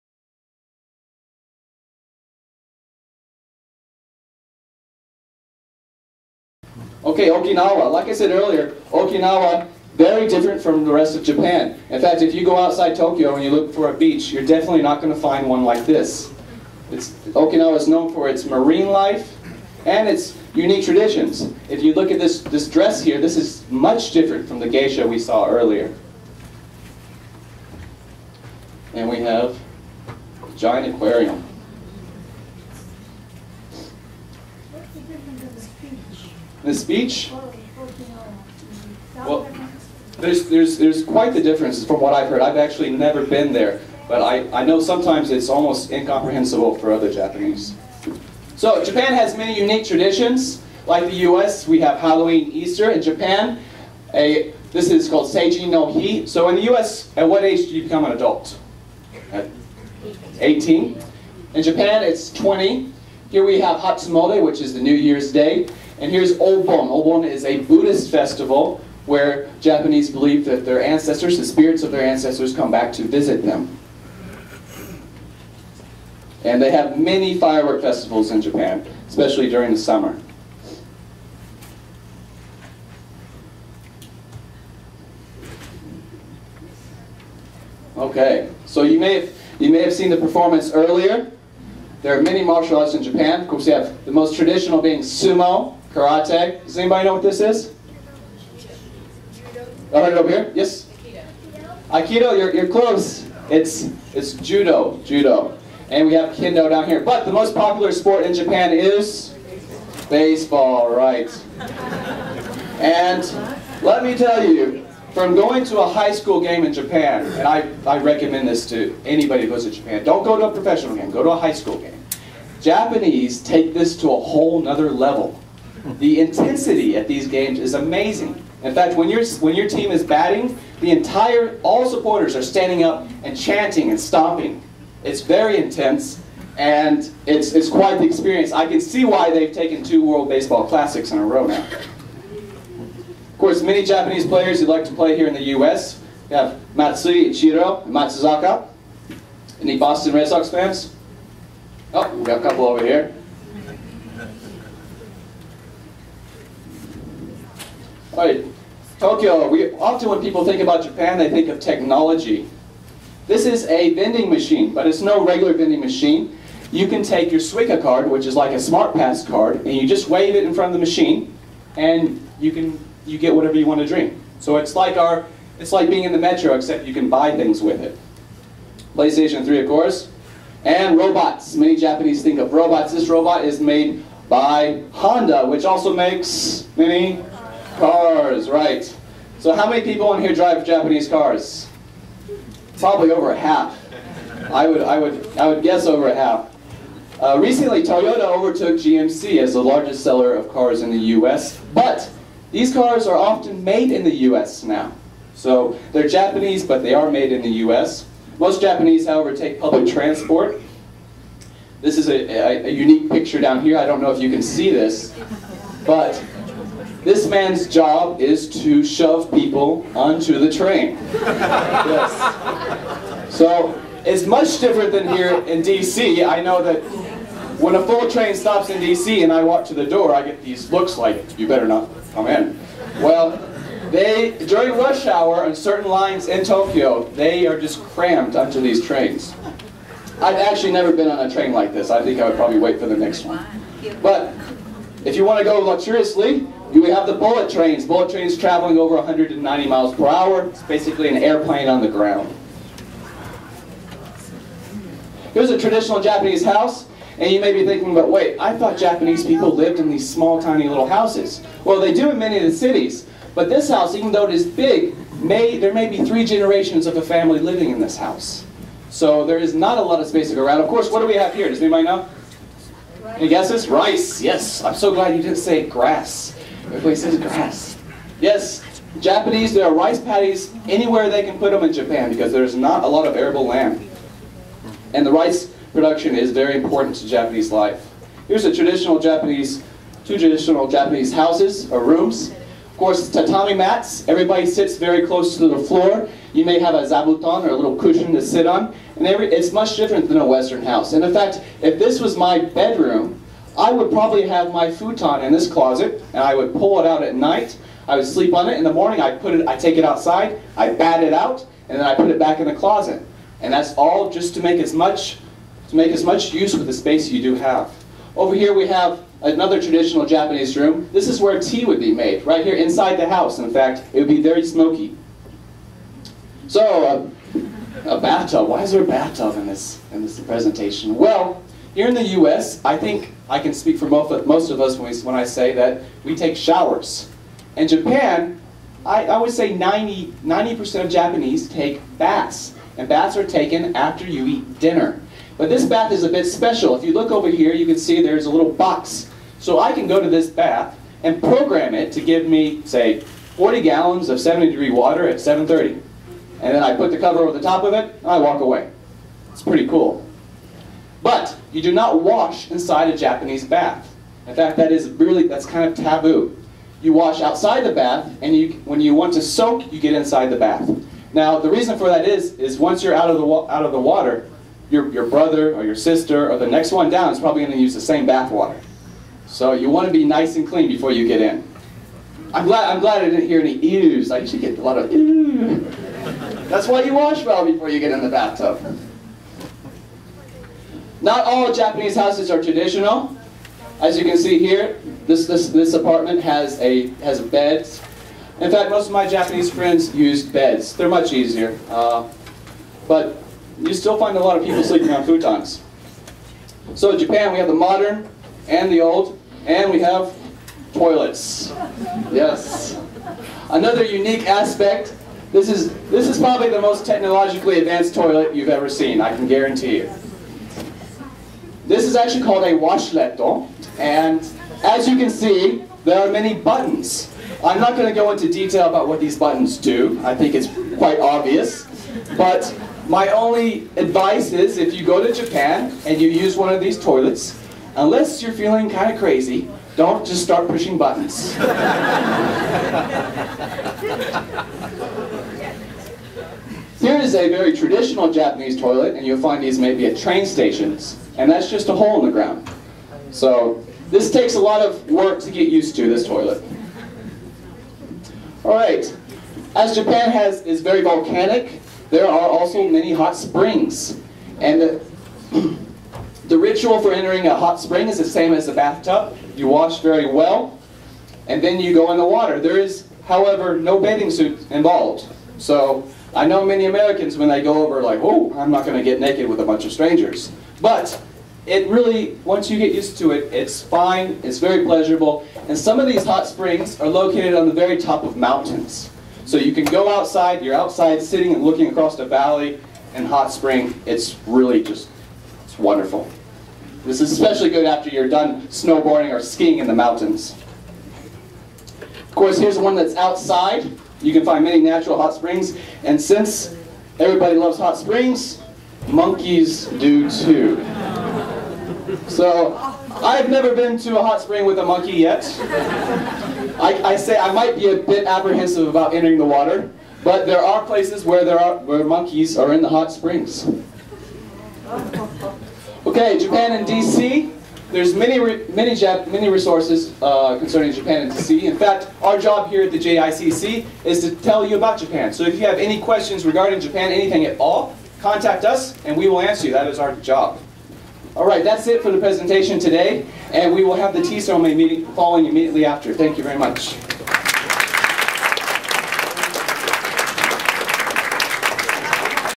Passport DC 2009 - Presentation, Part 2
Around 1600 people showed up to discover and celebrate Japanese culture as part of the 2009 Around the World Embassy Tour, a Passport DC event held each year in May. The Embassy of Japan had decided to hold that year's tour at the JICC, so guests could be treated to a tour of the gallery, Japanese snacks, martial arts demonstrations, and a tea ceremony performed by the Chado Urasenke Tankokai, a Washington DC Tea Society. You can watch the JICC's presentations on Japan and tea and the Urasenke's ceremony in this video. Join us next year for more fun.